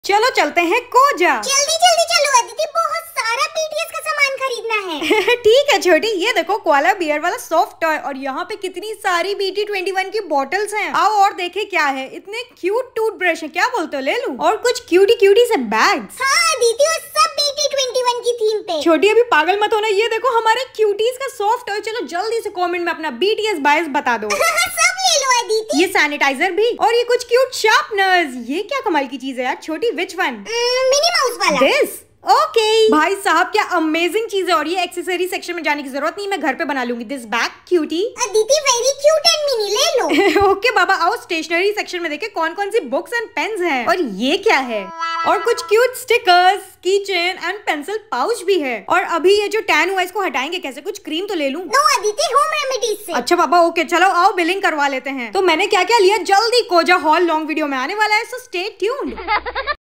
Haul - BT21 Koja 😱
I've started my series of Mini vlogs. I went to Koja store and shopped my heart out.

Do recreate my DIYs and share them with me on my Instagram Page and get featured.